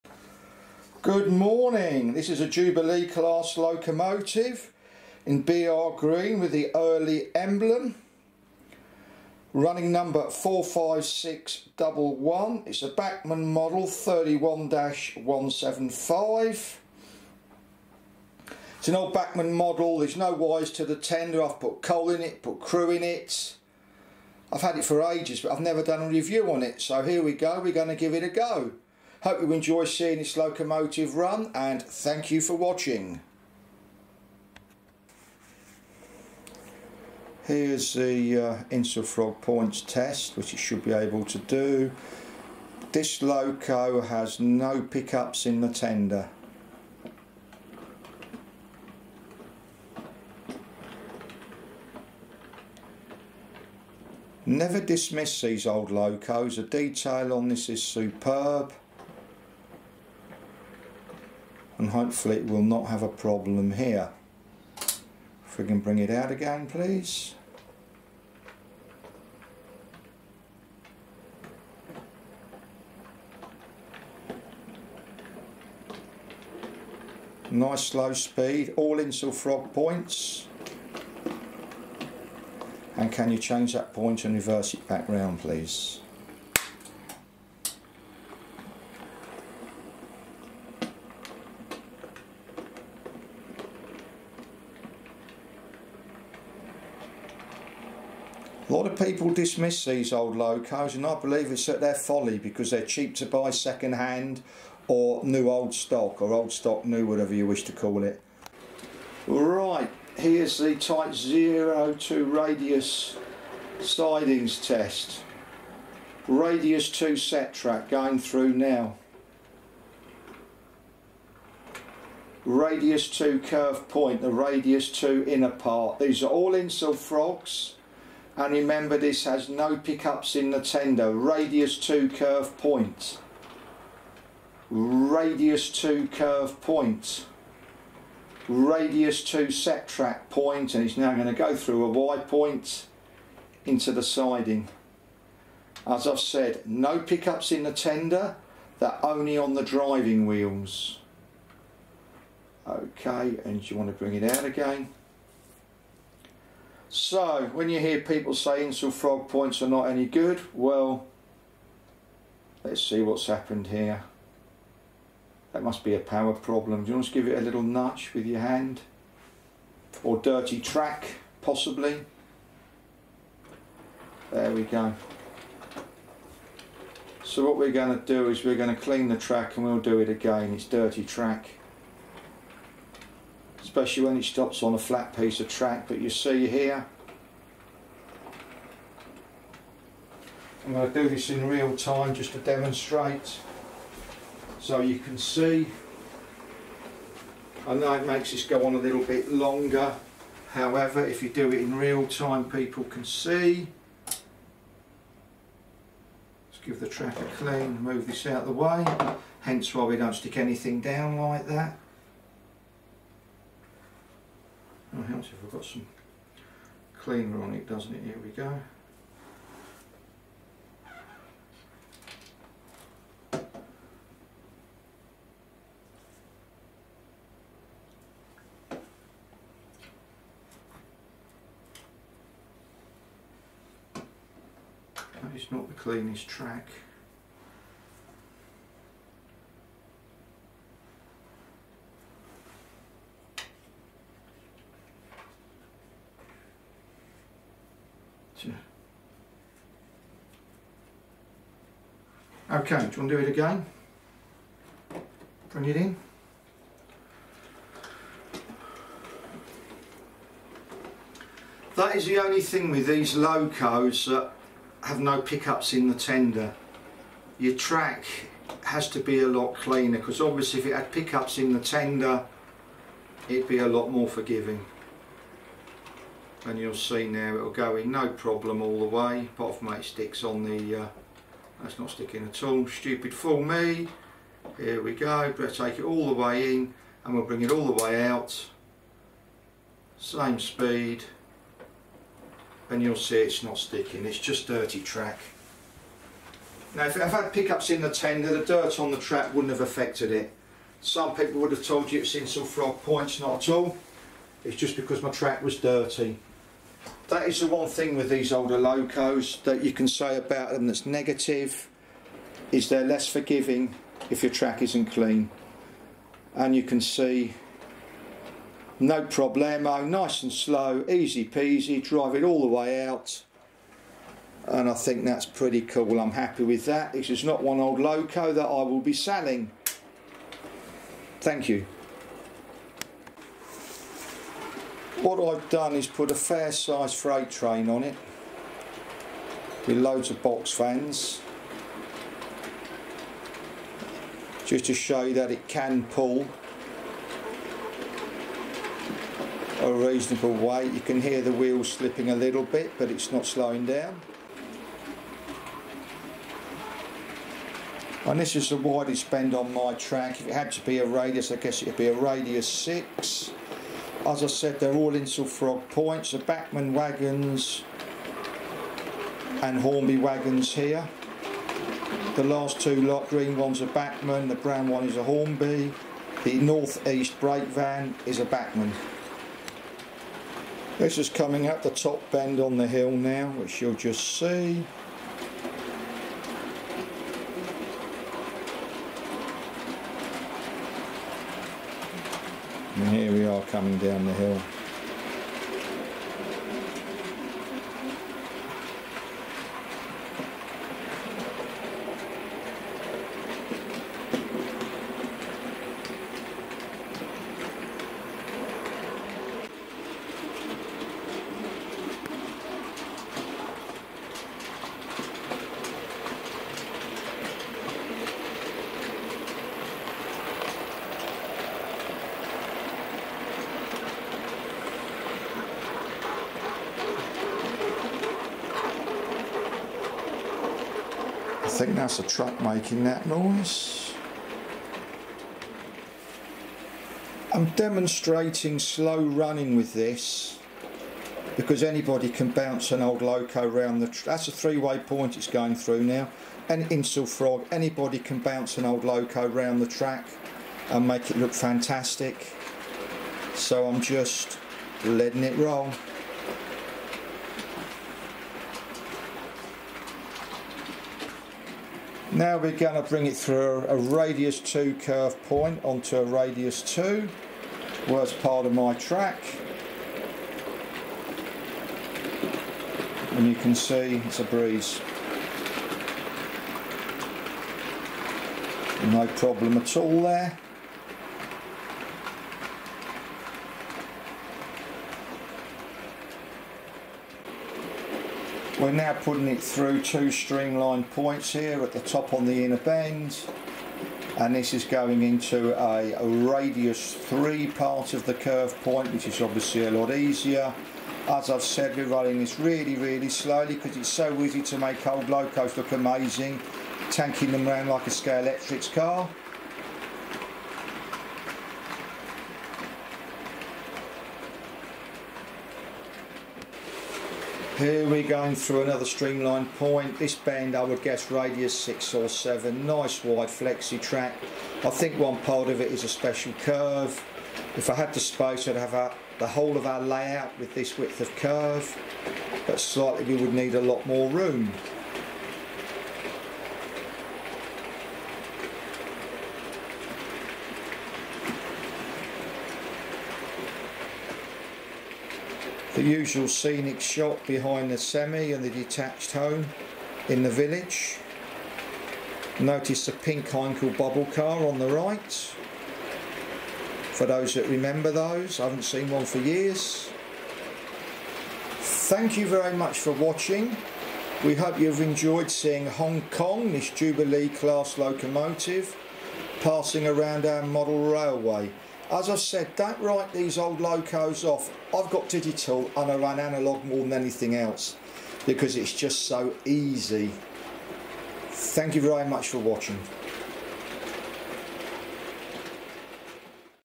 Jubilee Class locomotive "Hong Kong" review : Widescreen 4K
Bachmann 31-175, a Jubilee Class locomotive called "Hong Kong" review on all different types of track, all over insulfrog points. Being an older loco it does not have pick ups in the tender, only on the main driving wheels.